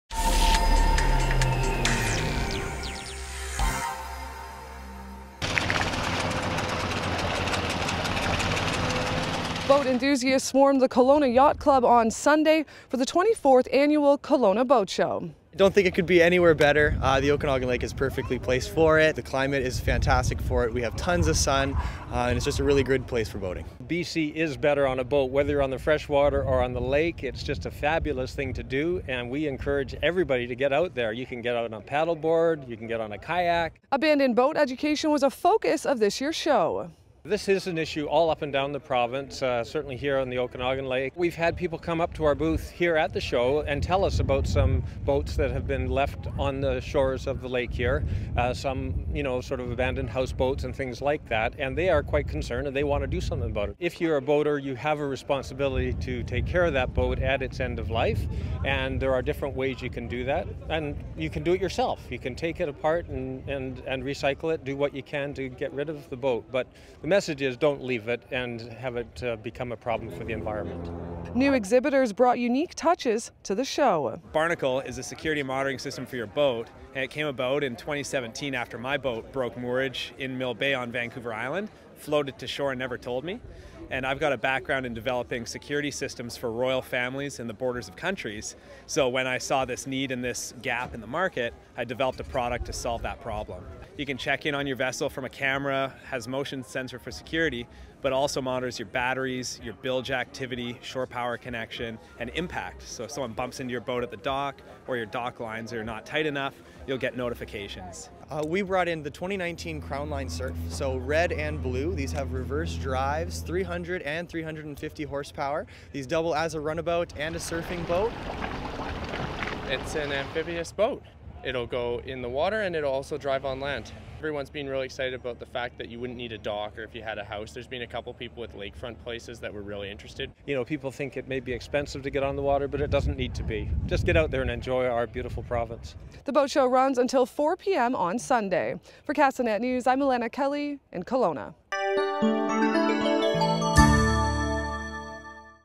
B.C. is better on a boat
Boat enthusiasts swarmed the Kelowna Yacht Club for the 24th annual Kelowna Boat Show this weekend.